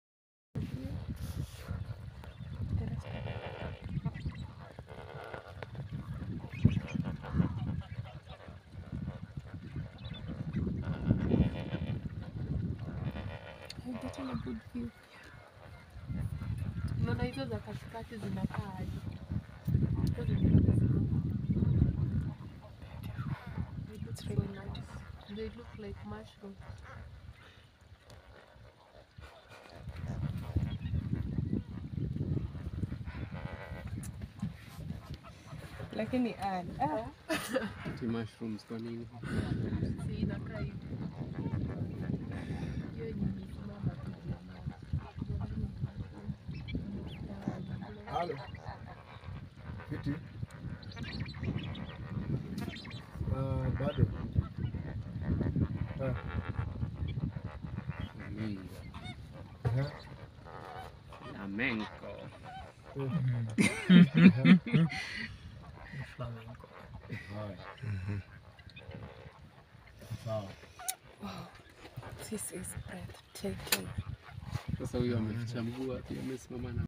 Beautiful Lake Amboseli and Flamingos | Lake Amboseli National Park
Most travelers visit Amboseli because of the elephants against the backdrop of Mt. Kilimanjaro. But you can also find Flamingos at Lake Amboseli.

This is Lake Amboseli, after which Amboseli National Park is named; and is usually an empty expanse of volcanic soil. Even during the wettest years its depth rarely exceeds two feet and this creates a mirage of a shimmering lake making the horizon appear fluid.

The parched dry lake bed is offset by the contrasting lush green vegetation in the marsh and swamp areas which are created by underground springs rising to the surface through the porous soil.